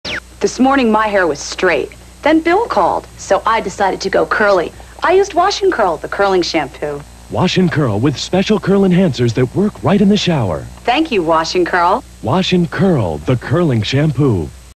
1992 Wash and Curl Shampoo "The Curling Shampoo" TV Commercial
The commercial was originally aired on December 8th, 1992. It was shown during the TV Movie "A Killer Among Friends". It was broadcast on WFSB Channel 3 in Hartford New Haven CT.